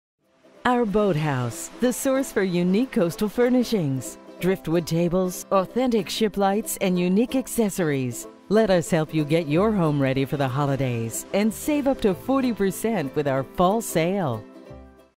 Our Boat House Coastal Home Furnishings
Home furnishings inspired by the sea! Seaglass greens, watery blues and sandy neutrals